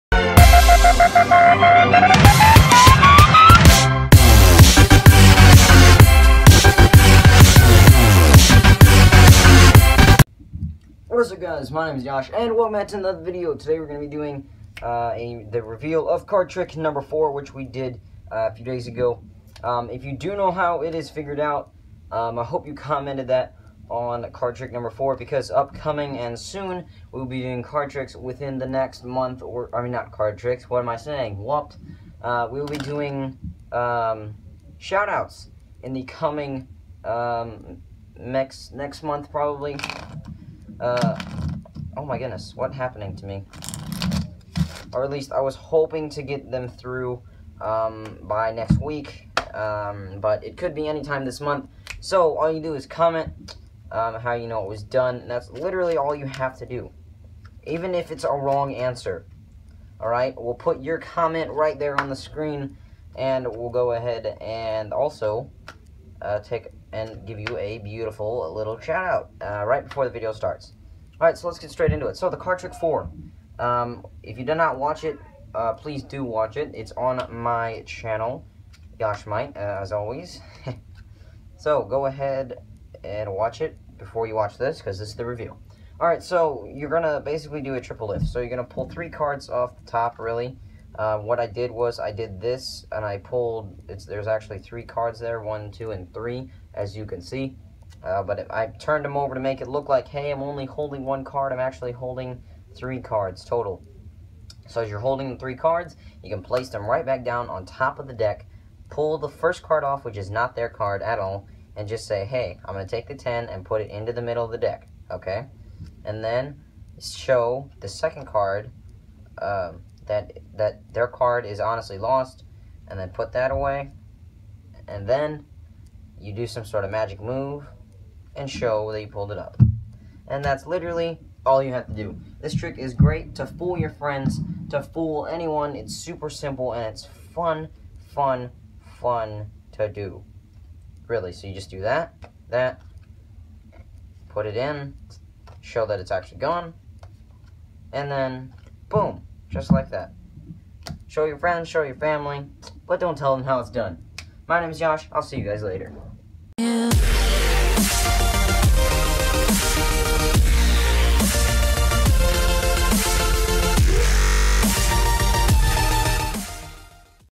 Card Trick #4 Revealed
Hey guys! My name is Yosh and I am a card teacher and creator or magic! I also do gaming and app showcases, hope you enjoy!

In this video I show the reveal to the previous card trick, #4. This trick is a fun little trick to pull on your friends as long as you do it right!